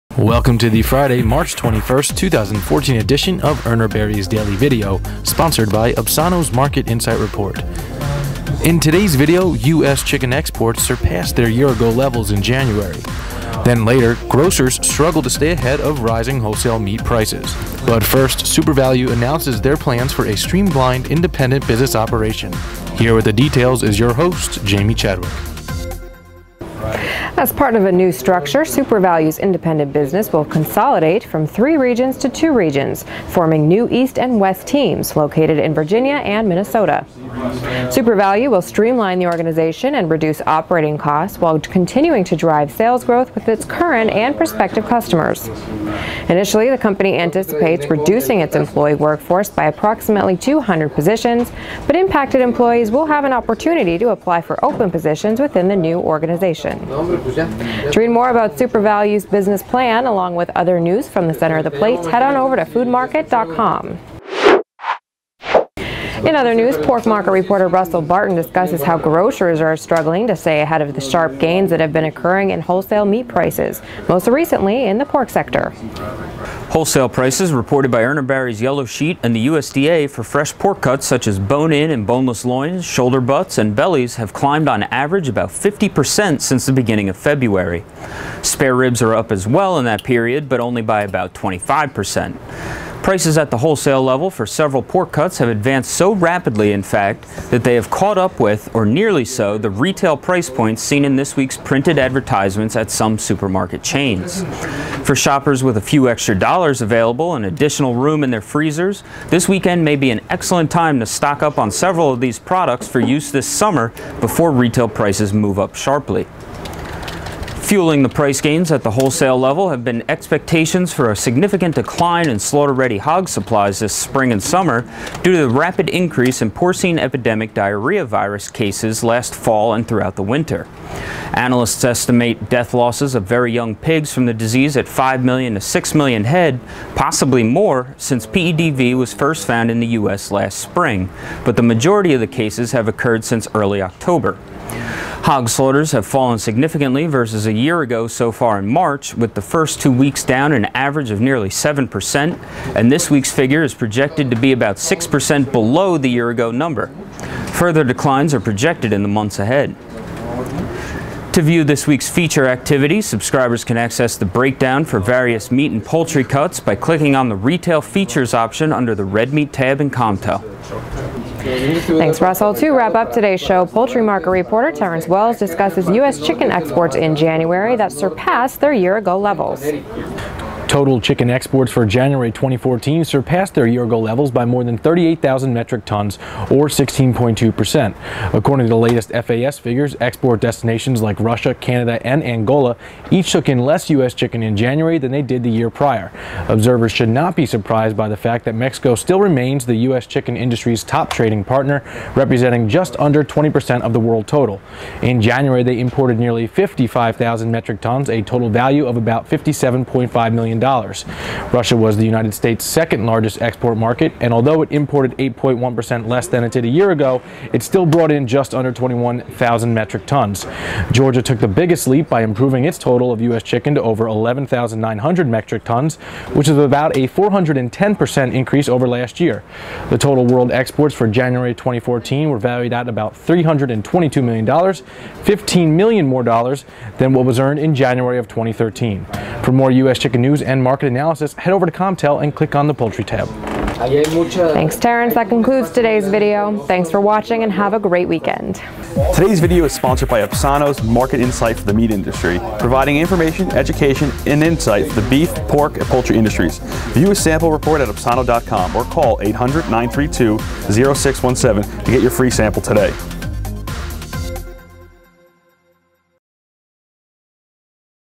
US Chicken Exports; Rising Wholesale Meat Prices; SUPERVALU Business Operation Plan
US Chicken exports surpass their year ago levels in January; Grocers struggle to stay ahead of rising wholesale meat prices; SUPERVALU announces their plans for a streamlined independent business operation; Sponsored by Obsono's Market Insight Report